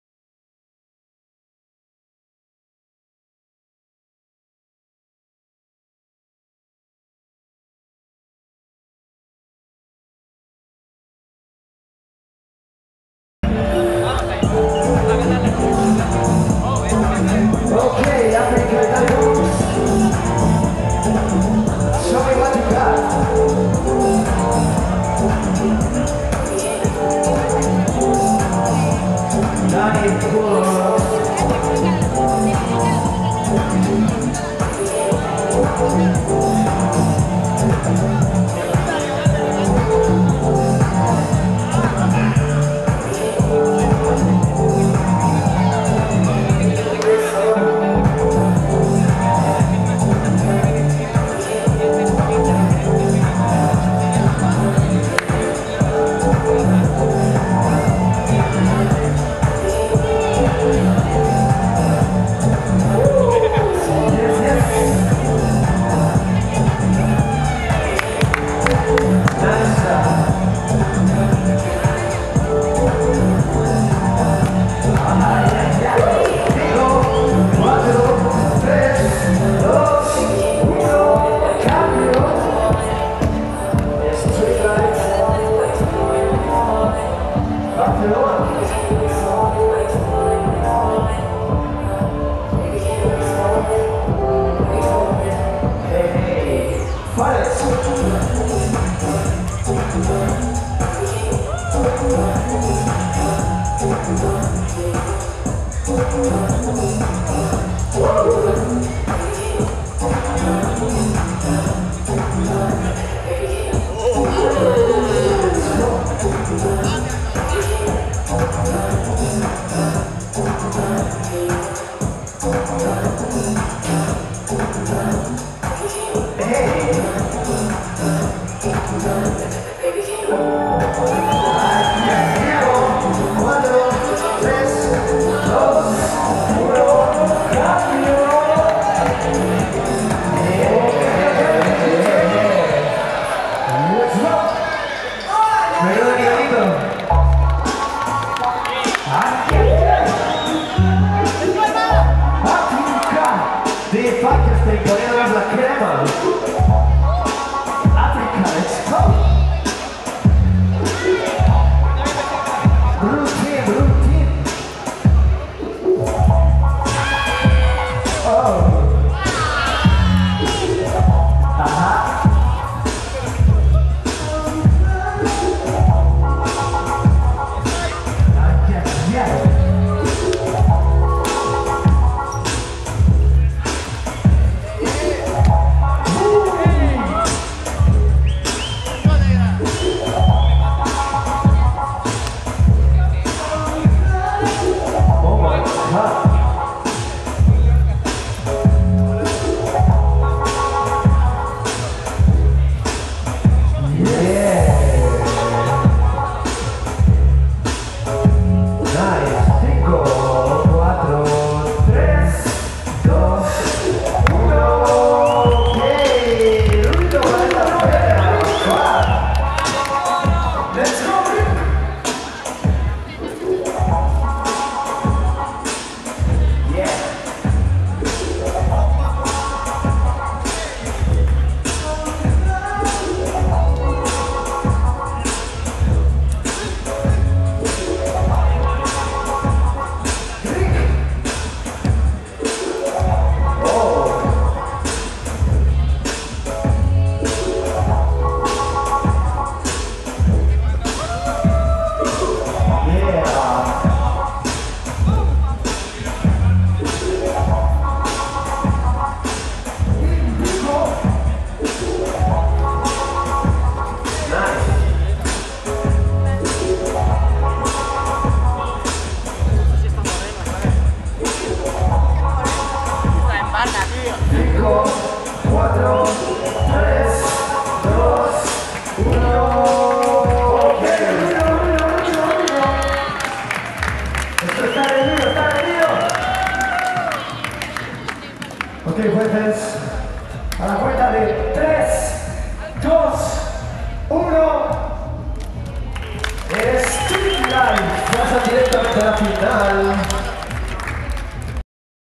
SEMIFINAL ALL STYLES 2VS2 URBAN SUMMER 2017 | LAREDO | BLABELCOMPANY
URBAN SUMMER EVENTO ORGANIZADO POR TRAP AVENUE

FILMED BY BLACK LABEL COMPANY